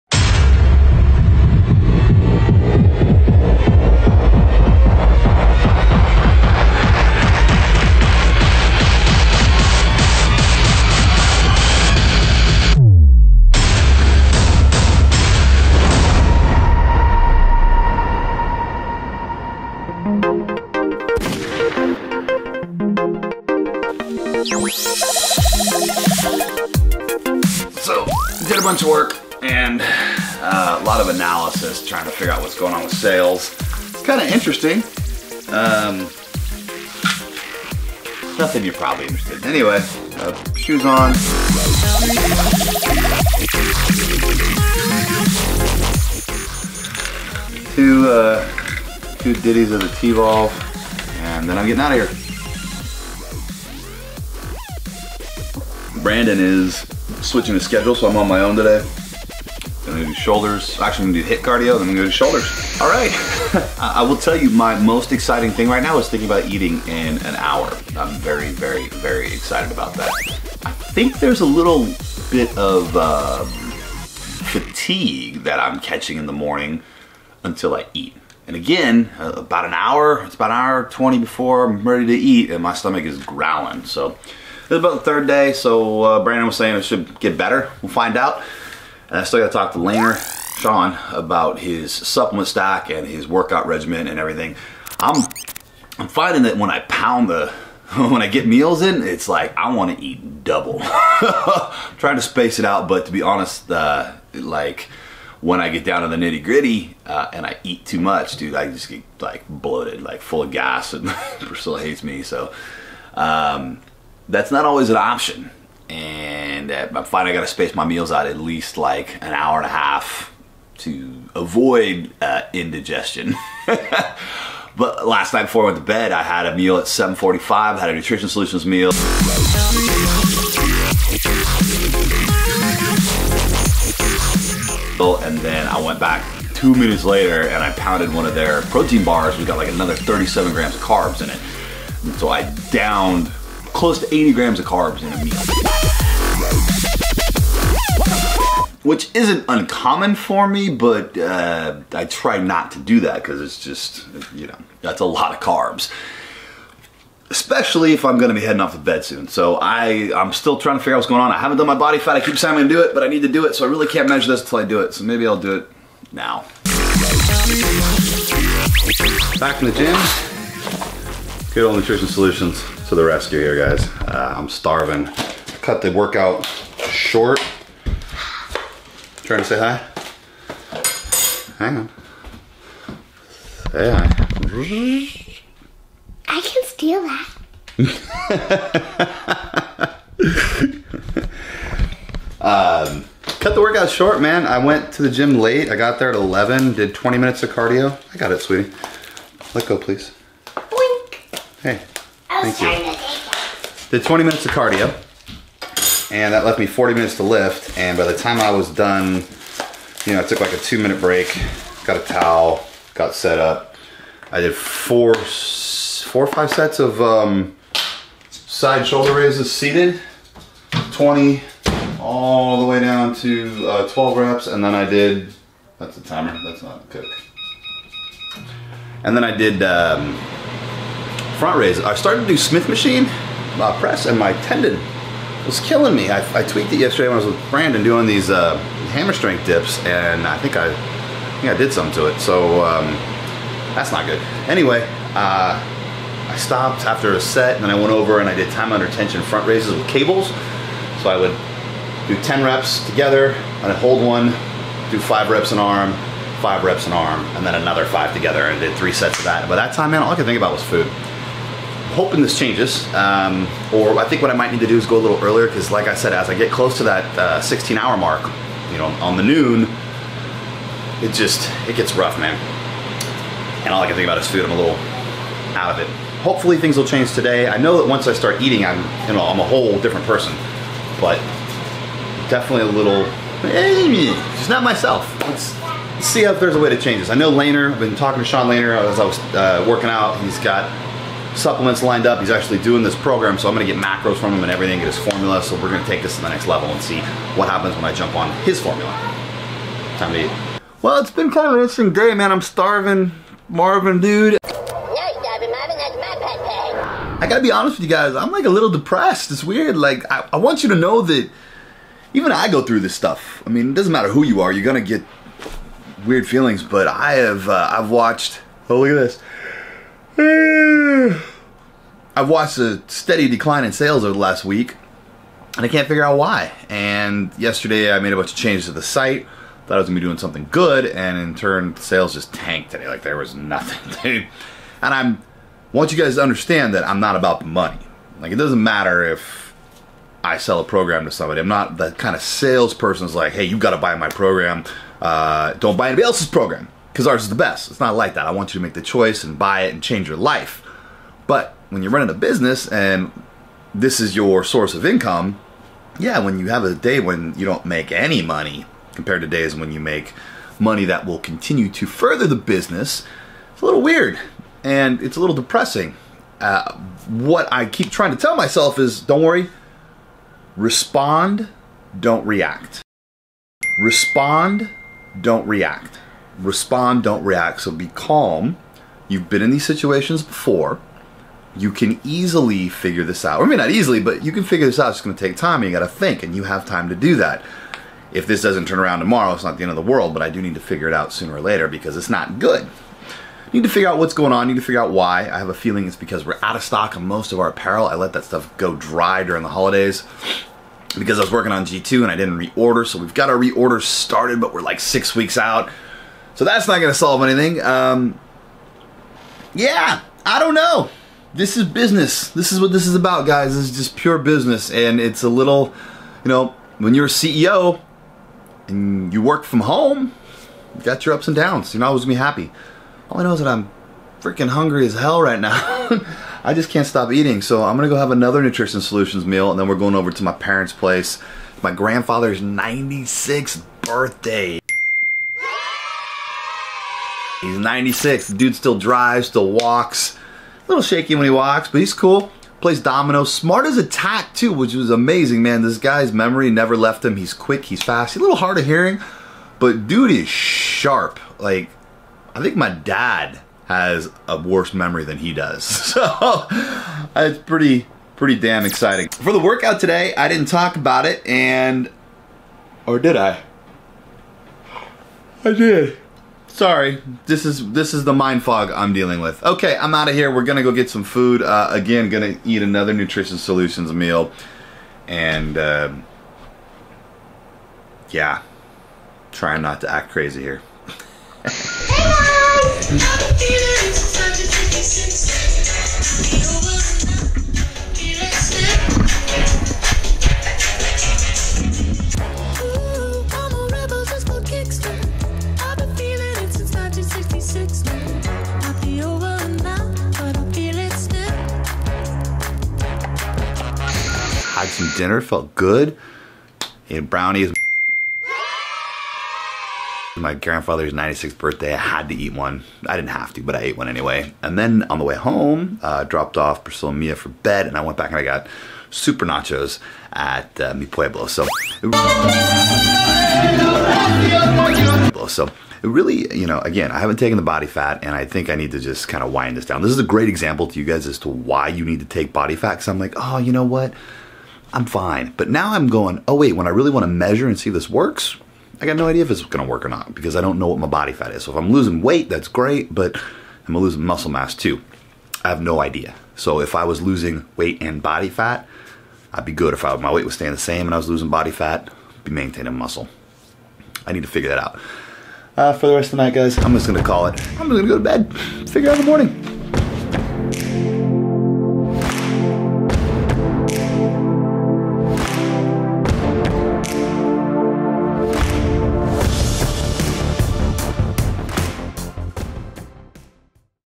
Body Spartan Raw Episode 4 | Behind the scenes
Day 4 of the intermittent fasting experiment. Gabe continues to power through day 4 of intermittent fasting with a fasted cardio session and a fasted shoulder workout. Strength is down, but so is body fat! Get the inside scoop on what it's like to actually do an intermittent fasting program as Gabe tells exactly how he's feeling, whether or not he's hungry and/or progressing.